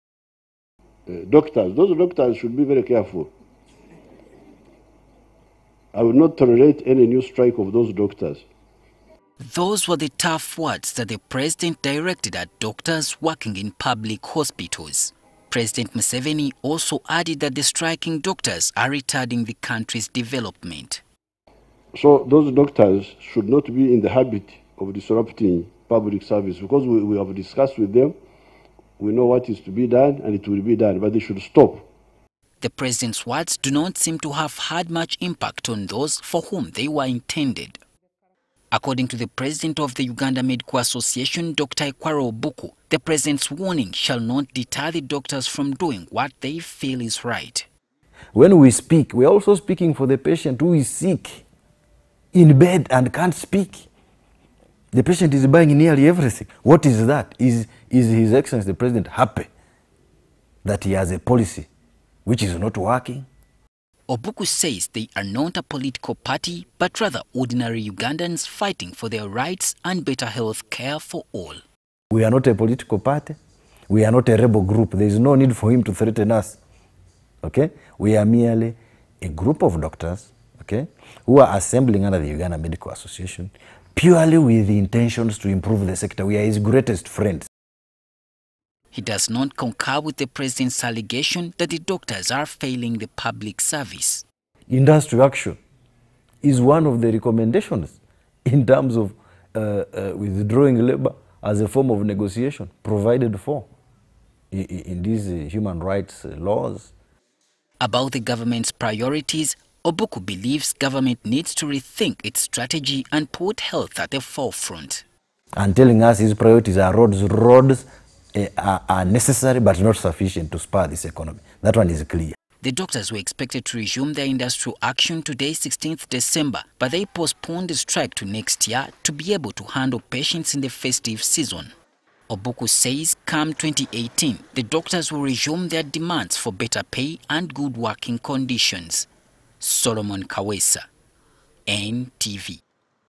DOCTORS’ STRIKE: Doctors respond to president’s threats
Doctors who work in public hospitals have scoffed at remarks made by the president claiming they will not capitulate to the whims and fancies of the Executive
In his speech yesterday during Thanksgiving prayers at State House Entebbe, President Yoweri Museveni said government will no longer tolerate striking civil servants
However, the president the Uganda Medical Association, Dr. Ekwaro Obuku, says they will not relent from pursuing their rights and a better healthcare system.